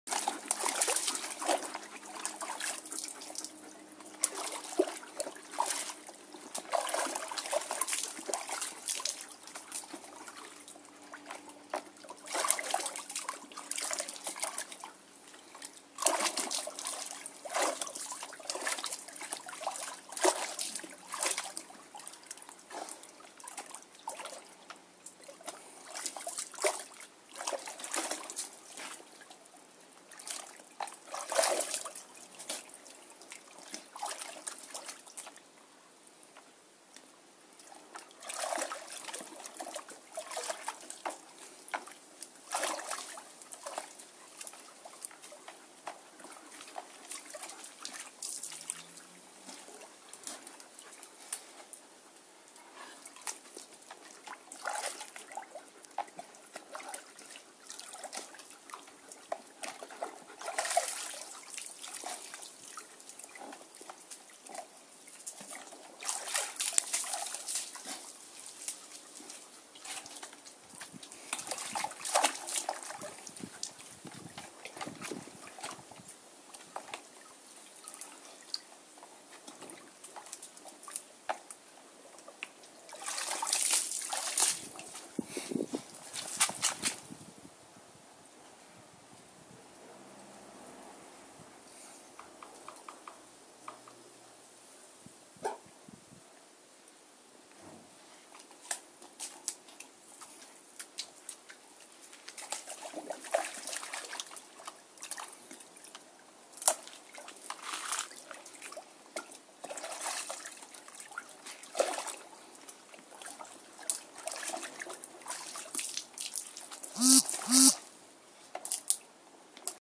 Boogie speelt balletje balletje in het badje
Boogie kan zich zo uren vermaken...